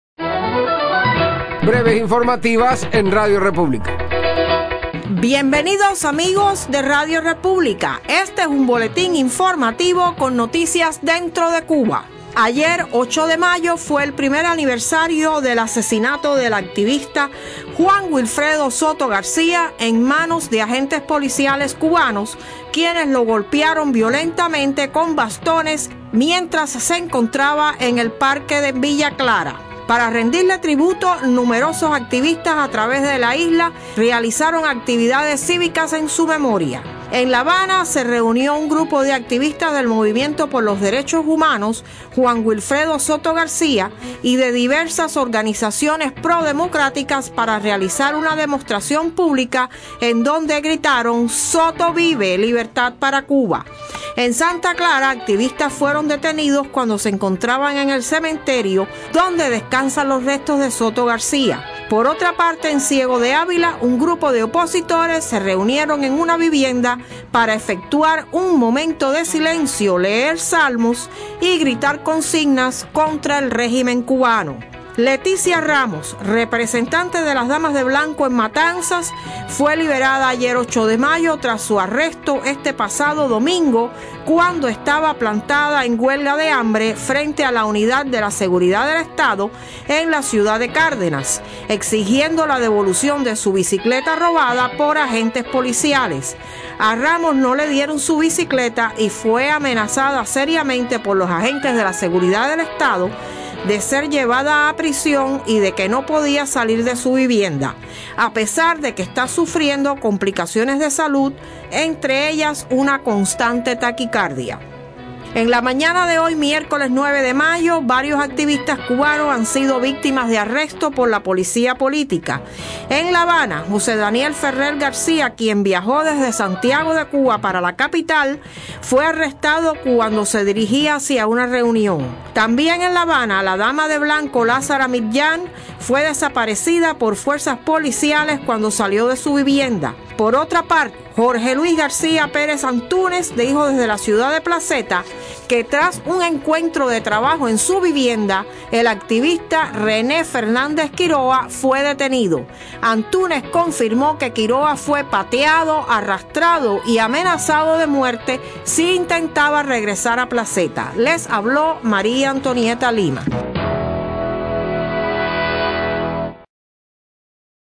Breve Informativo: Miercoles 9 de mayo del 2012
Miércoles 9 de mayo del 2012

Bienvenidos amigos de Radio República. Este es un boletín informativo
con noticias dentro de Cuba.

1.      Ayer, 8 de mayo fue el primer aniversario del asesinato del activista Juan Wilfredo Soto García en manos de agentes policiales quienes lo golpearon violentamente con bastones mientras se encontraba en un parque de Villa Clara.  Para rendirle tributo, numerosos activistas a través de la isla realizaron actividades cívicas en su memoria.  En la Habana, se reunió un grupo de activistas del Movimiento por los Derechos Humanos Juan Wilfredo Soto García y de diversas organizaciones pro-democráticas para realizar una demostración pública en donde gritaron "¡Soto Vive!" y "¡Libertad para
Cuba".    En Santa Clara, activistas fueron detenidos cuando se encontraban en el cementerio donde descansan los restos de Soto García.  Por otra parte, en Ciego de Ávila un grupo de opositores se reunieron en una vivienda para efectuar un momento de silencio, leer salmos, y gritar consignas contra el régimen cubano.

2.      Leticia Ramos, representante de las Damas de Blanco en Matanzas, fue liberada ayer, 8 de mayo, tras su arresto este pasado domingo cuando estaba plantada en huelga de hambre frente la Unidad de la Seguridad del Estado en la ciudad de Cárdenas, exigiendo la devolución de su bicicleta robada por agentes policiales.  A Ramos no le dieron su bicicleta y fue amenazada seriamente por agentes de la Seguridad del Estado de ser llevada a prisión y de que no podía salir de su vivienda, a pesar de que esta sufriendo de compilaciones de salud, entre ellas una constante taquicardia.

3.      En la mañana de hoy, miércoles 9 de mayo, varios activistas cubanos han sido victimas de arrestos por la policía política.  En la Habana, José Daniel Ferrer García, quien viajo desde Santiago de Cuba para la capital, fue arrestado cuando se dirigía hacia una reunión.  También en la Habana, la Dama de Blanco Lazara Mitjan fue desaparecida por fuerzas policiales cuando salio de su vivienda. Por otra parte, Jorge
Luis García 'Antúnez' dijo desde la ciudad de Placetas que, tras un encuentro de trabajo en su vivienda, el activista Rene Fernández Quiroa fue detenido.  Antúnez confirmo que Quiroa fue pateado, arrastrado y amenazado de muerte si intentaba regresar a Placetas.